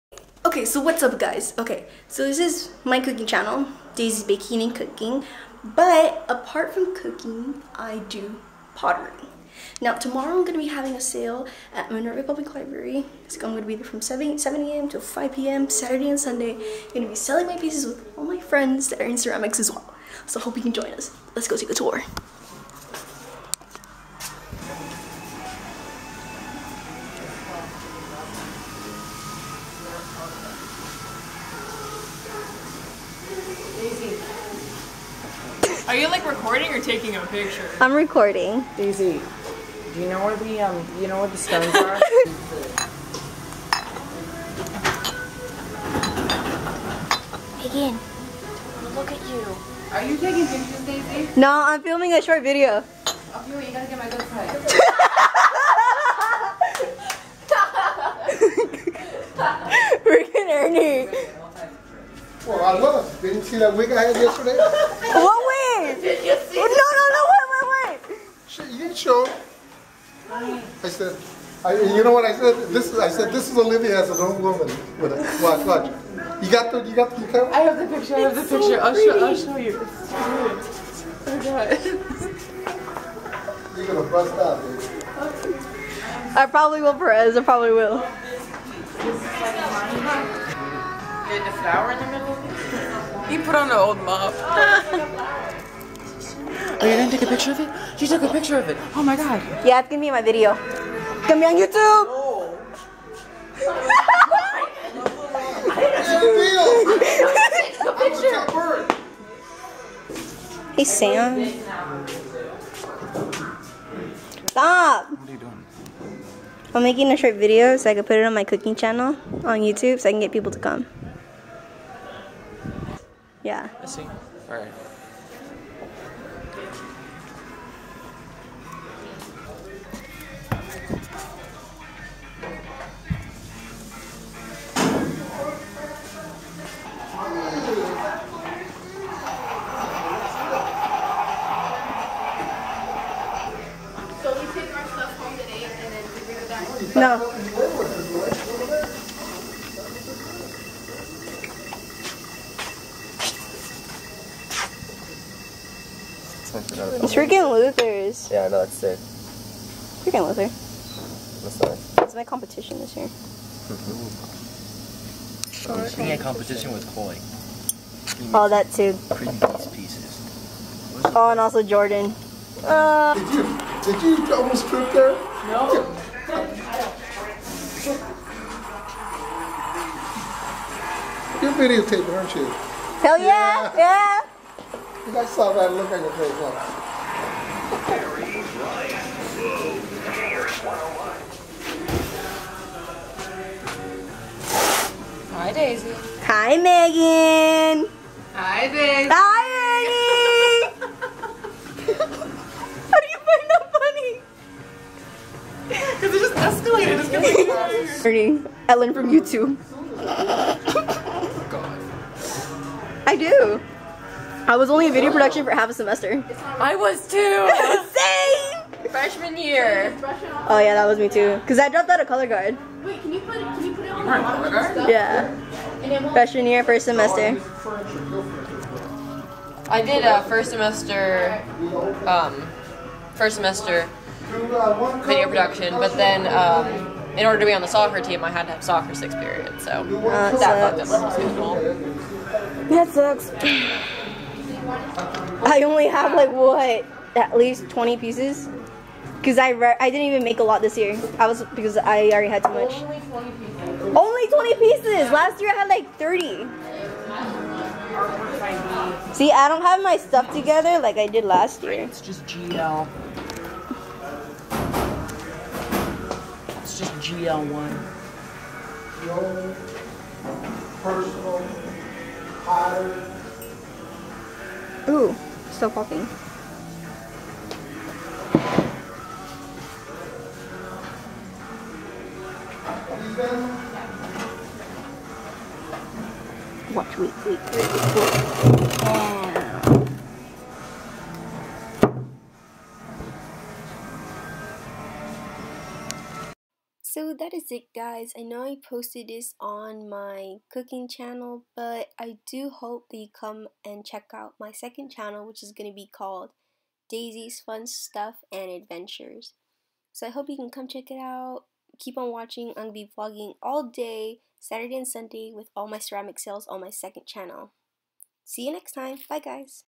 My Ceramics Class
Sorry guys this isn't a new cooking video but I'm working on it. But I am Happy to say that I have a second channel now !
At my High School, we have a ceramics class which I have been in for almost 3 years. Every year we participate at a local arts festival. I will be vlogging all day Saturday and Sunday about my ceramics sale.